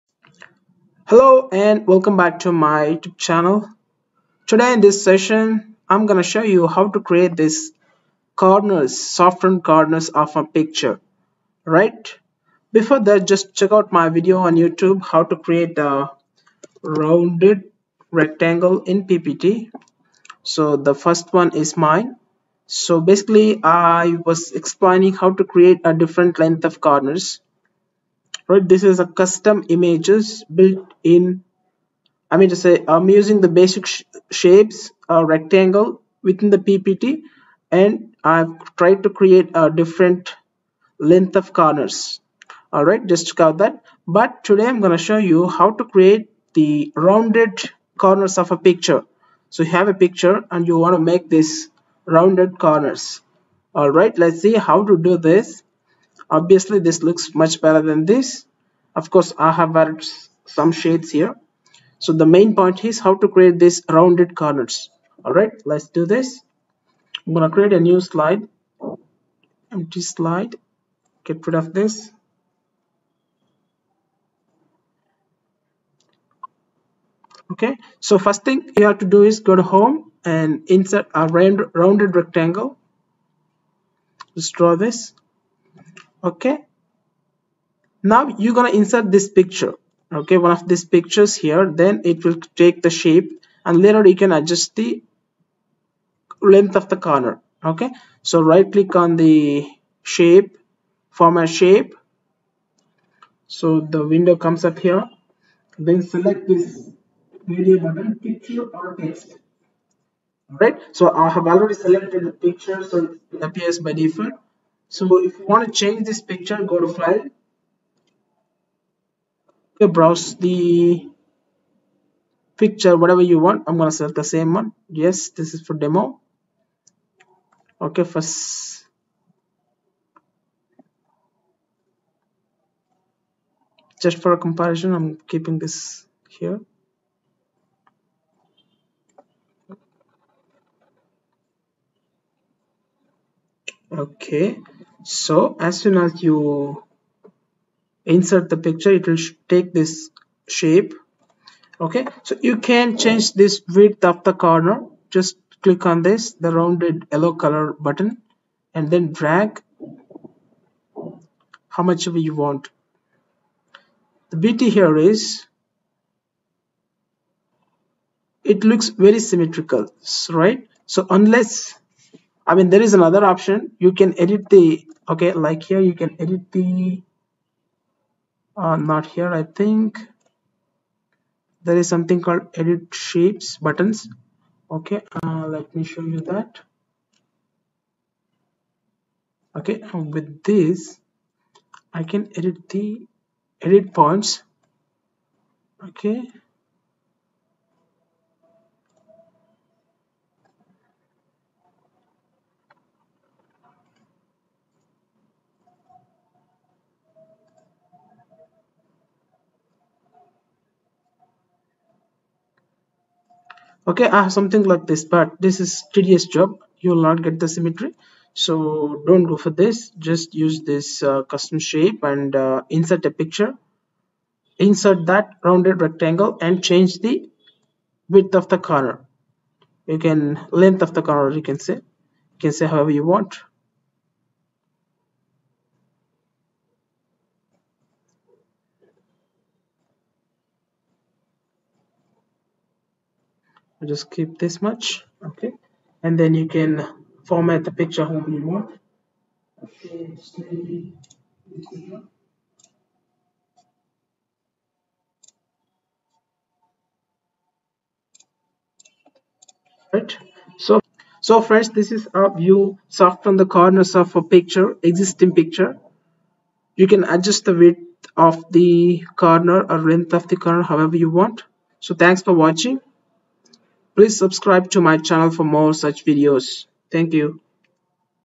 rounded corner picture in ppt
Create a rounded corner of a picture.

Insert a rounded corner rectangle from the Home menu.

Go to edit shape, then fill with a picture which should look with rounded corners.